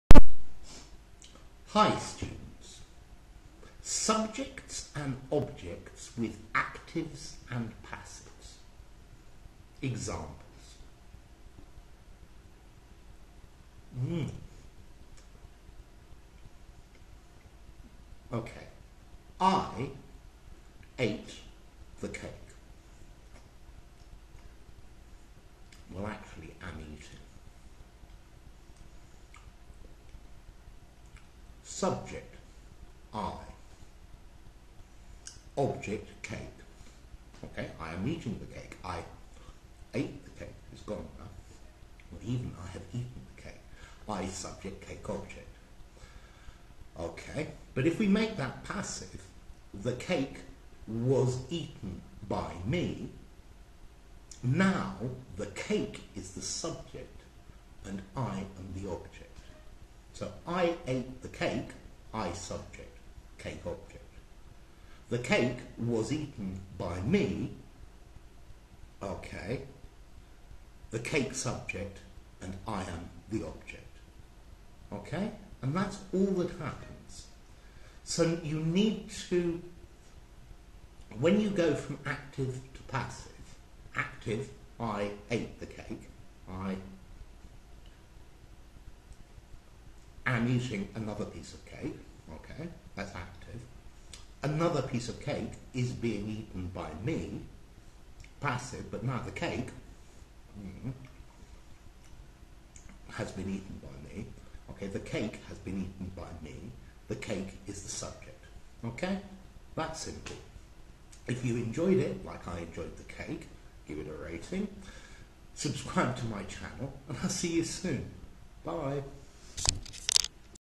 🔵 Subjects and Objects of Actives and Passives - ESL British English Pronunciation
A short explanation of subjects and objects with active and passive verbs